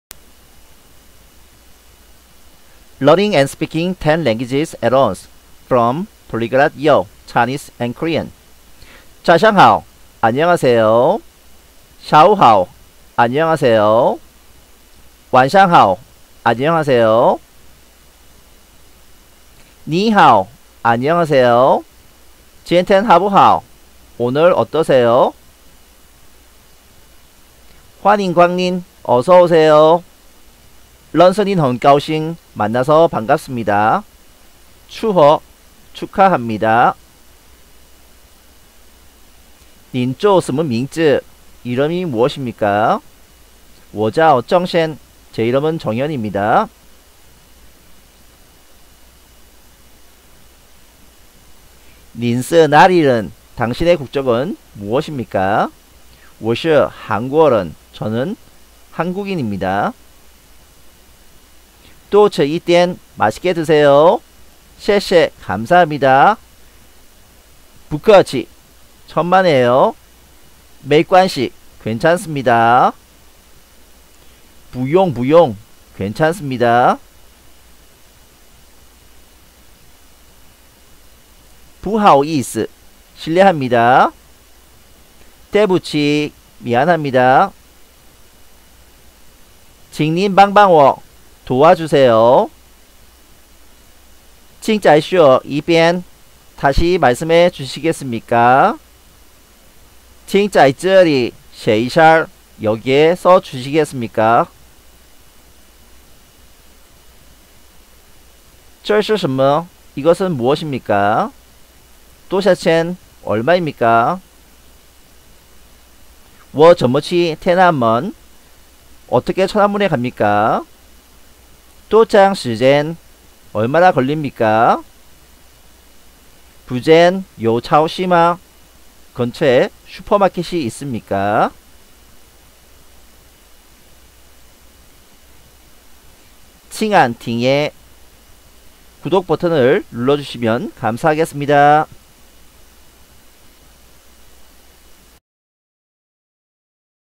[20개국어 배우기] Chinese&Korean 중국어&한국어 HyperPolyglot be Yourself 汉语&韩语 基础中文 韩语学习 세계일주용 중국어 급한분
早上好 안녕하세요(아침) Zǎoshang hǎo 下午好 안녕하세요(오후) Xiàwǔ hǎo 晚上好 안녕하세요 (저녁) Wǎnshàng hǎo
你好 안녕하세요? Nǐ hǎo 今天好不好?. 오늘 어떠세요? jīntiān hǎobù hǎo
欢迎光临 어서 오세요 Huānyíng guānglín 认识你很高兴 만나서 반갑습니다. Rènshì nǐ hěn gāoxìng 祝贺 축하합니다 Zhùhè
您叫什么名字 이름이 무엇입니까? Nín jiào shénme míngzì 我叫"正炫" 제 이름은 정현입니다. Wǒjiào  "Zhèng xuàn"
您是哪里人? 당신의 국적은 무엇입니까? Nín shì nǎlǐ rén? 我是韩国人 저는 한국인입니다. Wǒ shì hánguó rén
多吃一点 맛있게 드세요
Duō chī yī diǎn
谢谢 감사합니다. xiè xie
不客气 천만에요. Bù kèqì.
没关系 괜찮습니다. Méiguānxì.
不用不用 괜찮습니다. Bùyòng Bùyòng
不好意思 실례합니다.
bùhǎoyìsī
对不起 미안합니다.
Duìbùqǐ
请您帮帮我! 도와주세요
Qǐng nín bāng bāng wǒ
请再说一遍? 다시 말씀해주시겠습니까?
Qǐng zàishuō yībiàn
请在这里写一下?
여기에 써 주시겠습니까?
Qǐng zài zhèlǐ xiě yīxià
这是什么? 이것은 무엇입니까?
Zhè shì shénme
多少钱? 얼마입니까?
Dōshǎo qián
我怎么去天安门?
어떻게 천안문에 갑니까?
Wǒ zěnme qù tiān'ānmén
多长时间? 얼마나 걸립니까?
Duō cháng shíjiān
附近有超市吗？
근처에 수퍼마켓이 있습니까?
Fùjìn yǒu chāoshì ma?